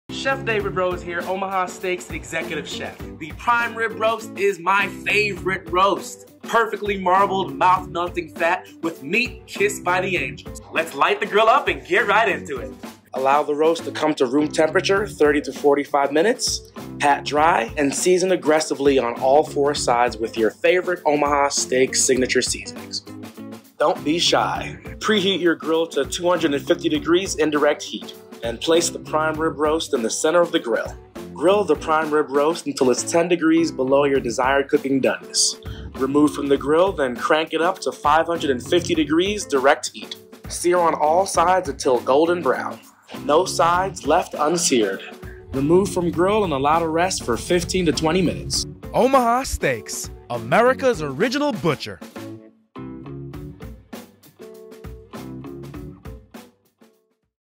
How to Grill a Prime Rib Roast
​The prime rib is my favorite roast and for good reason. It’s intensely marbled with bold, beefy flavor and has an irresistible tender juicy texture making it a classic holiday centerpiece. Follow my video as I show you how to grill a perfect prime rib roast. Let’s light up the grill!

1. Thaw completely and bring to room temperature
2. Season on all sides with your favorite Omaha Steaks seasoning or rub
3. Preheat grill to 250°F indirect heat and grill to 10° below desired doneness
4. Sear at 550°F direct heat - about 1 minute each side
5. Rest 15-20 minutes
6. Slice and serve with my easy shallot gravy